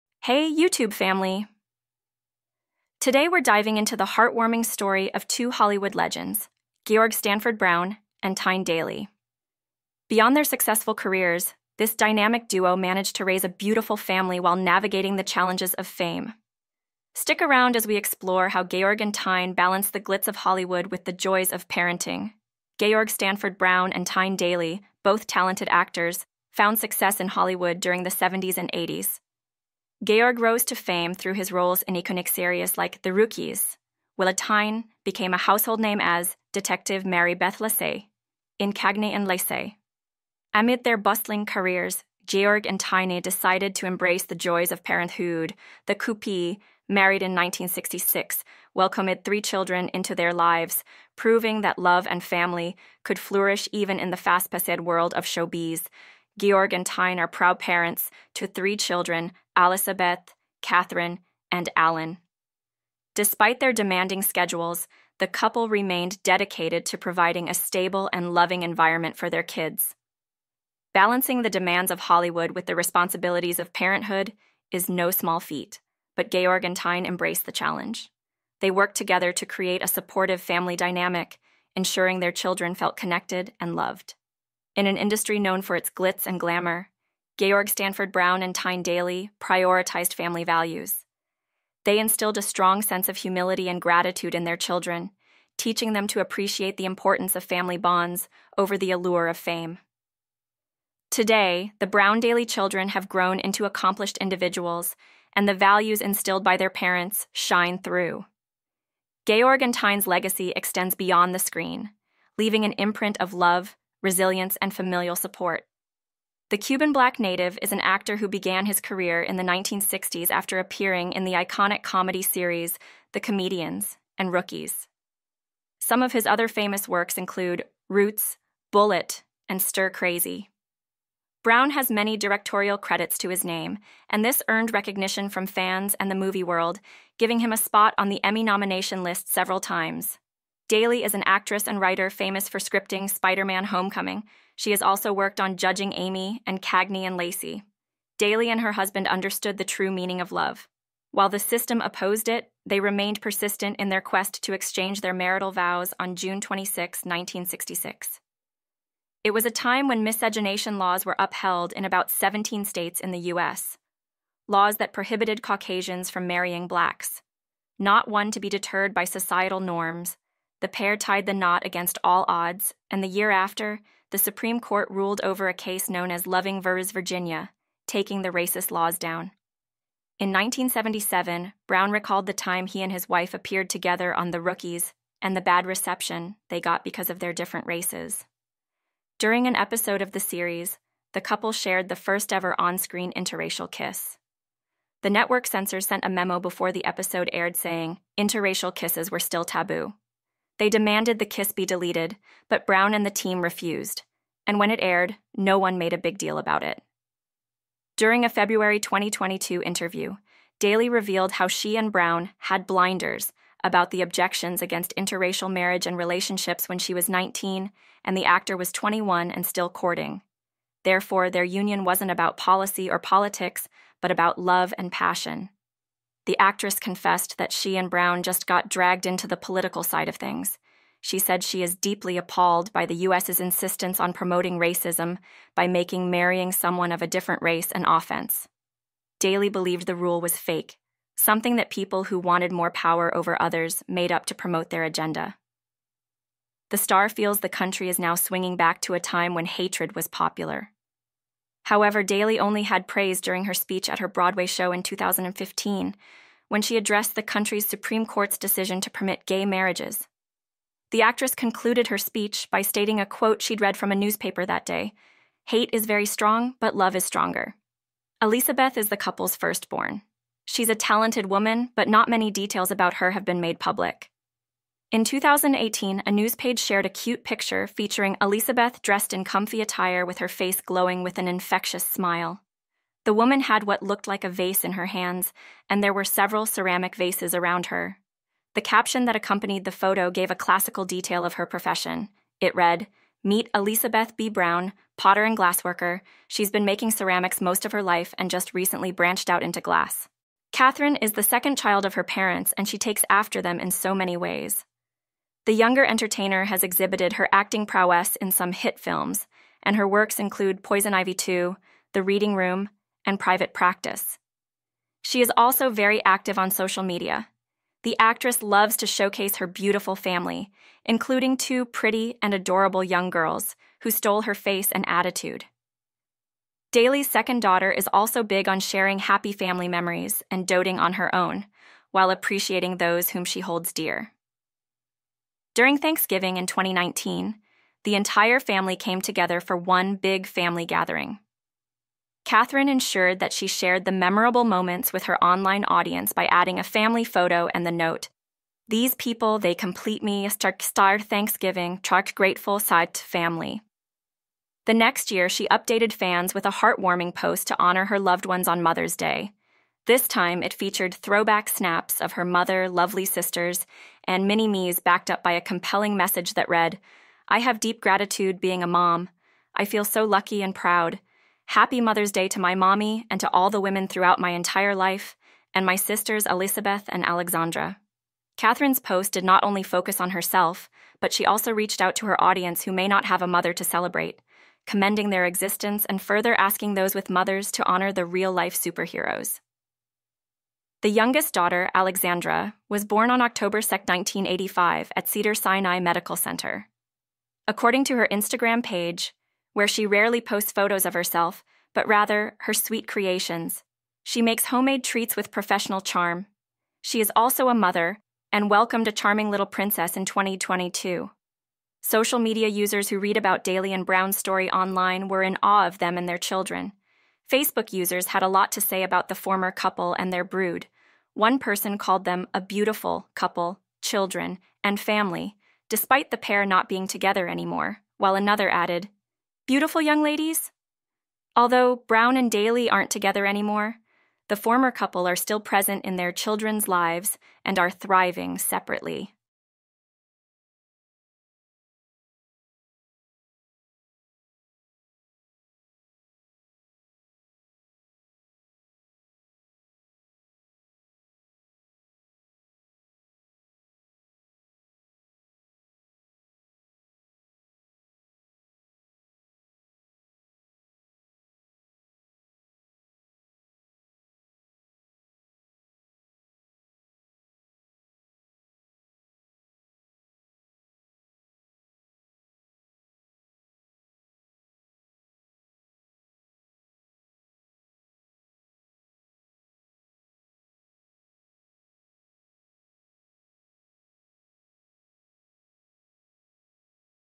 Georg Stanford Brown & Tyne Daly 3 Kids Are Grown Up Despite Racial Backlash — Pics of Daughters
Tyne Daly and her former husband, Georg Stanford Brown, were married for almost three decades before they went their separate ways. The legendary stars welcomed three daughters during their marriage, and they are now grown women with families.

 Georg Stanford Brown & Tyne Daly Raised 3 Kids despite Racial Backlash — Pics of Their Daughters